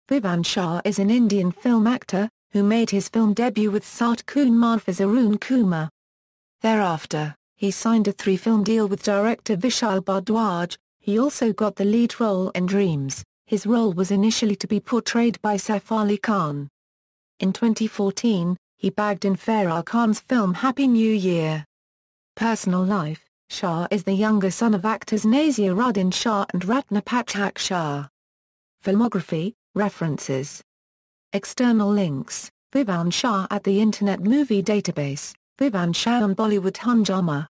Vivaan Shah
Vivaan Shah (born 11 January 1990) is an Indian film actor, who made his film debut with Saat Khoon Maaf (2011) as Arun Kumar. Thereafter, he signed a three-film deal with director Vishal Bhardwaj, he also got the lead role in Dreams, His role was initially to be portrayed by Saif Ali Khan. In 2014, He bagged in Farah Khan's film Happy New Year.